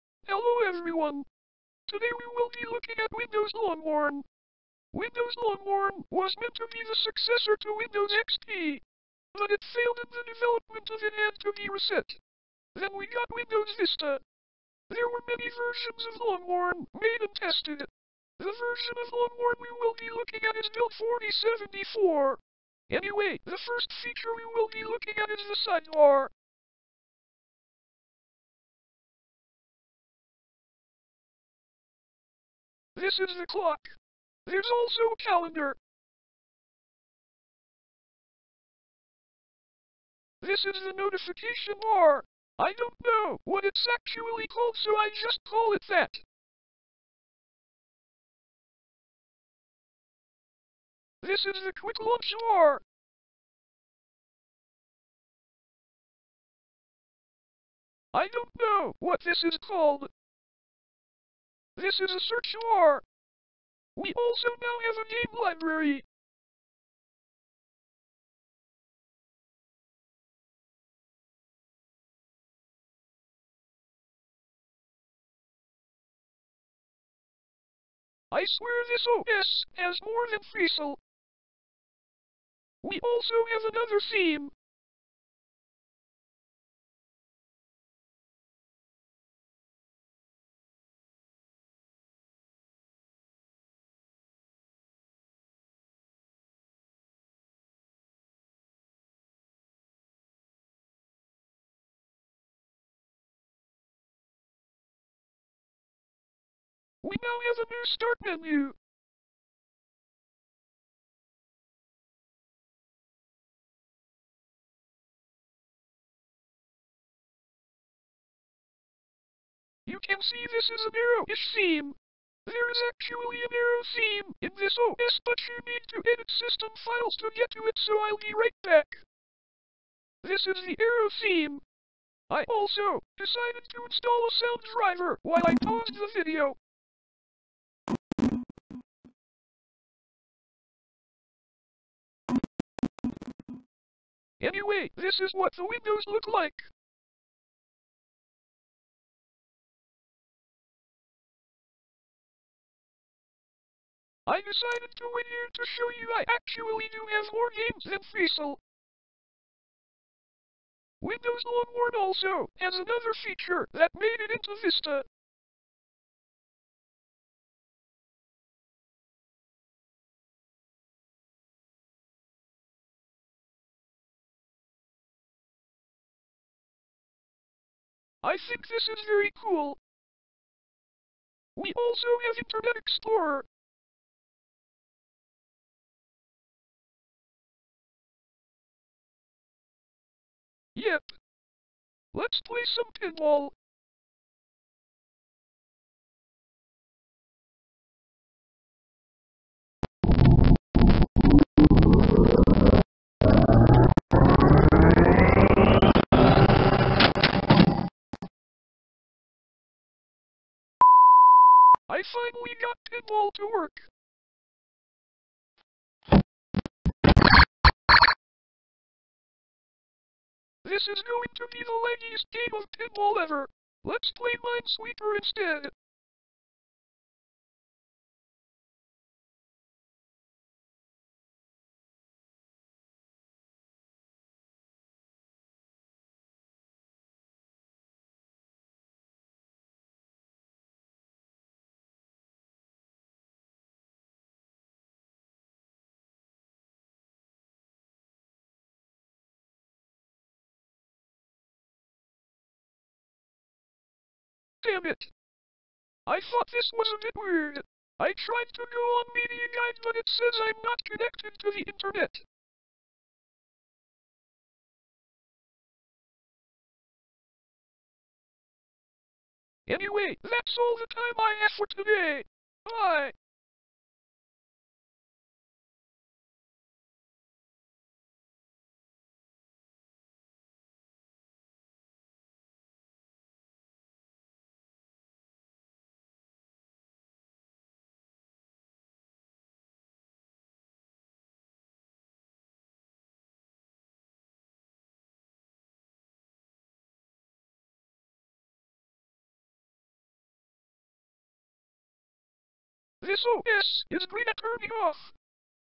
Windows Longhorn Build 4074
Today we will be looking at what could technically be called a never released operating system.